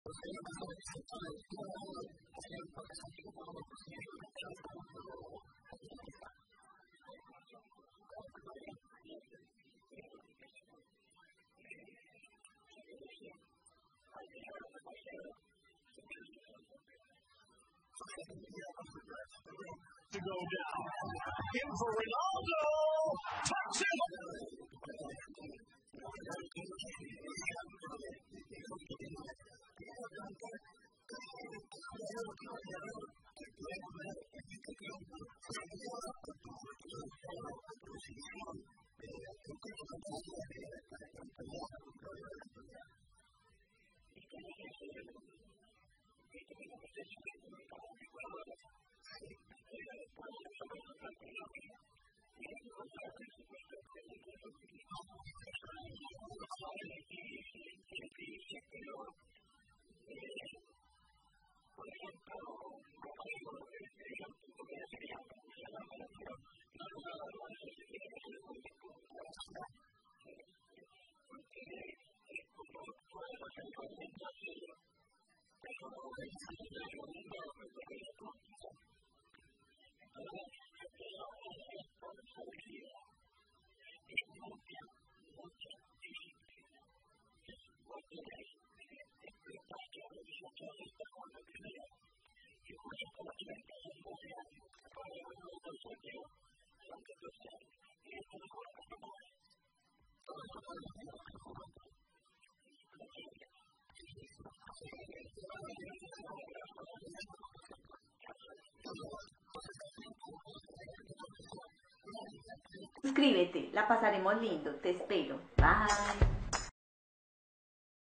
San Filippo : ningun jugador actual es el 50% de lo que fui yo, una inderacta a Messi y Cr7
San Filippo : ni jugador actual es el 50% de lo que yo fui yo les tiro una inderecta a Messi y Cristiano Ronaldo